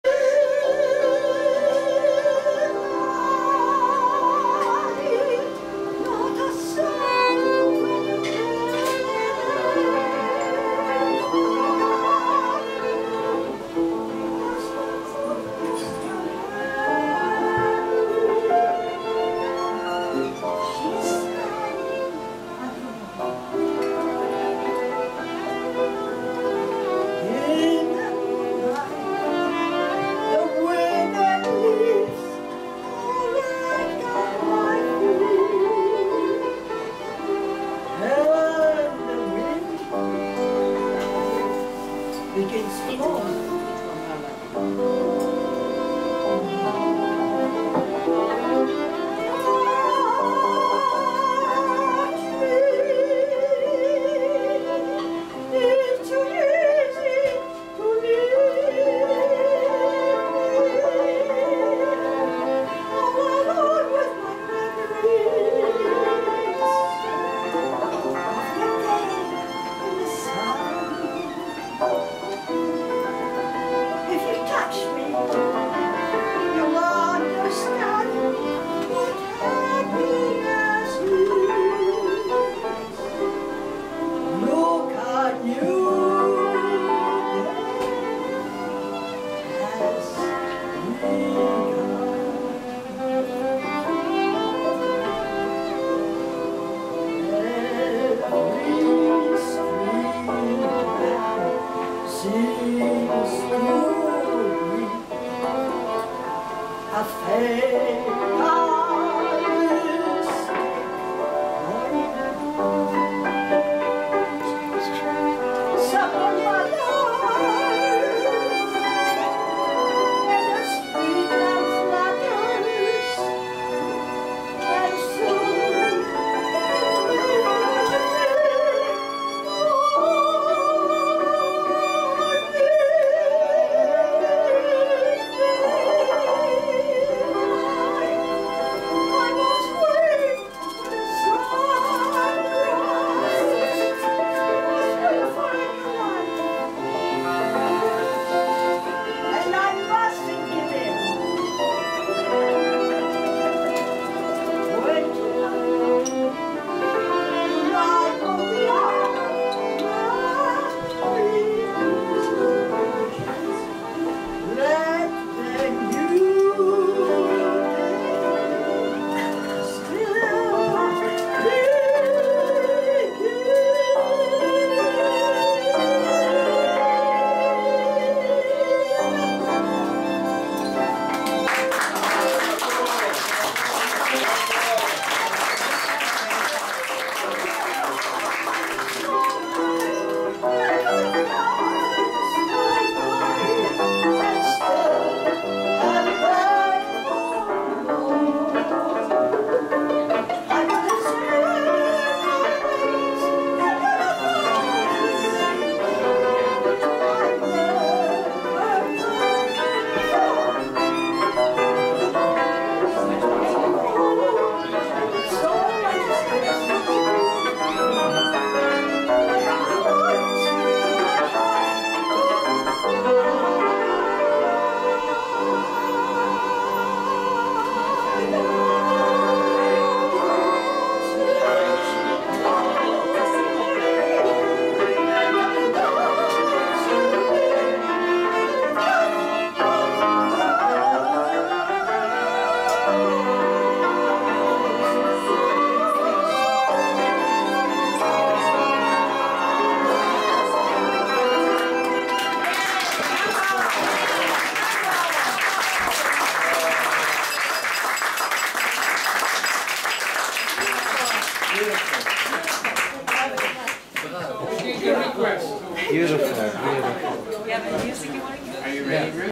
Fôkus : Hongrois-Budapest 2015-06-27 5/6
Montréal - Plateau-Mont-Royal
La Galerie Fôkus
Isabella  -  singer - chanteuse
Istvan Lakatos  -  violin / violon
Josef Marko  -  Zymbalon